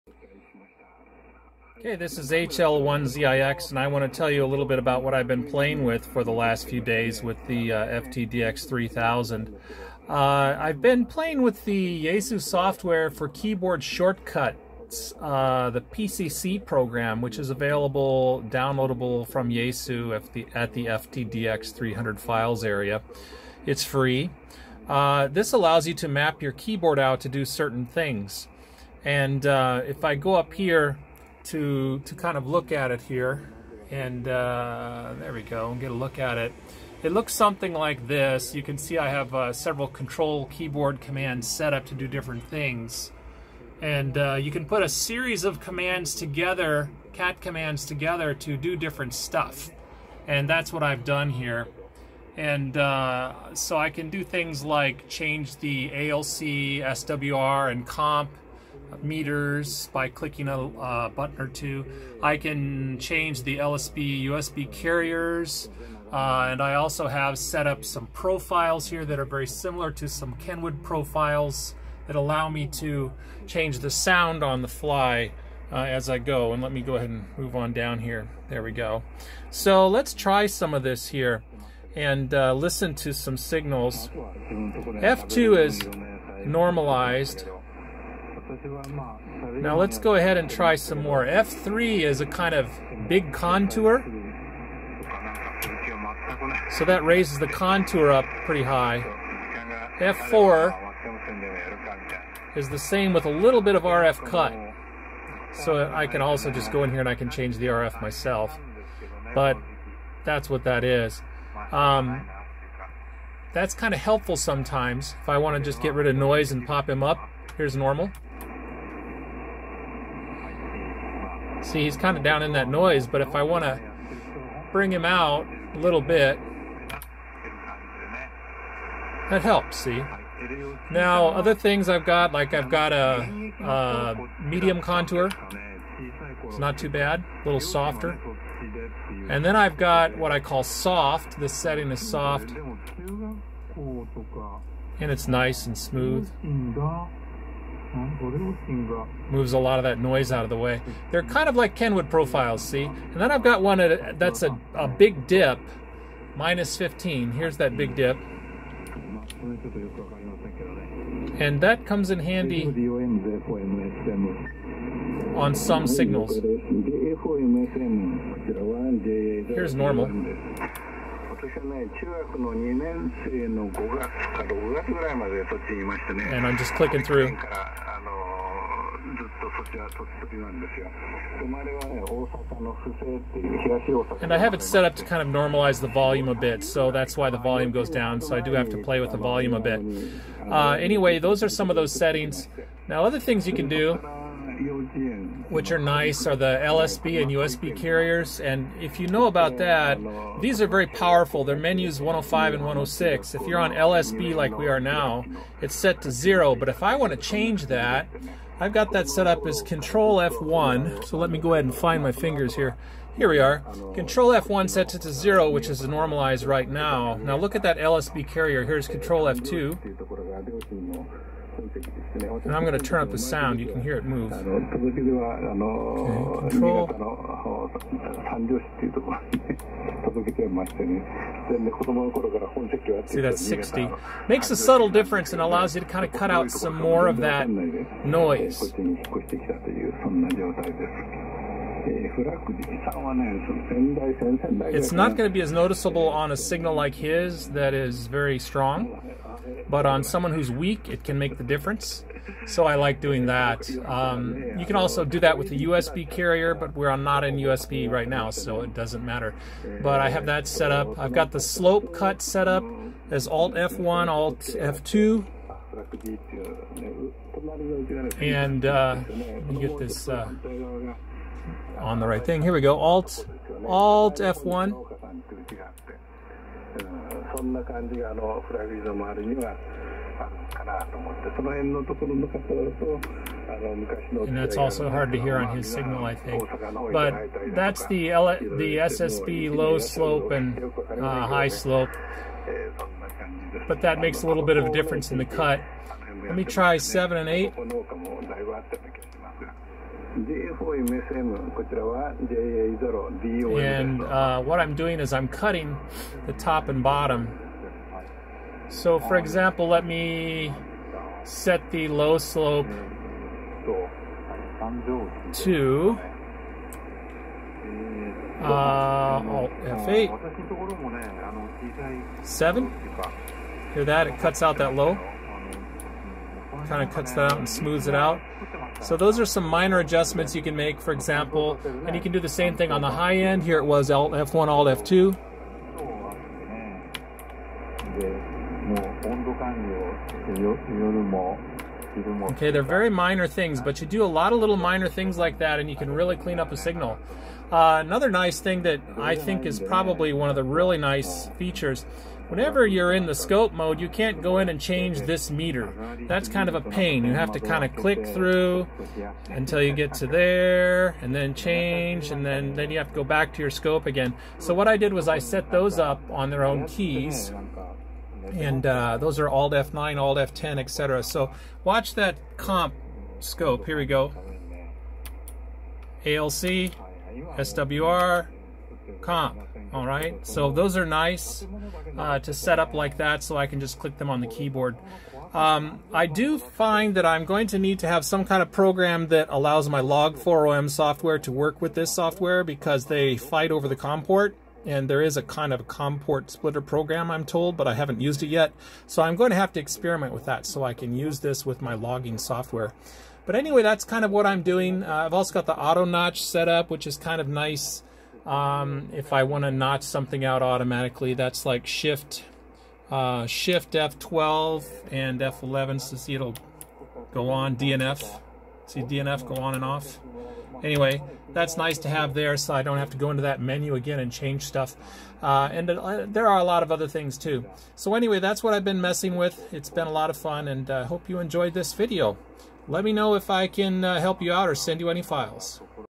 FTDX3000 and Yaesu PCC CAT Software
I mapped some CAT key commands to my Yaesu FTDX3000, using the Yaesu PCC software.  This software, and manual, are available free from Yaesu's FTDX3000 downloads page.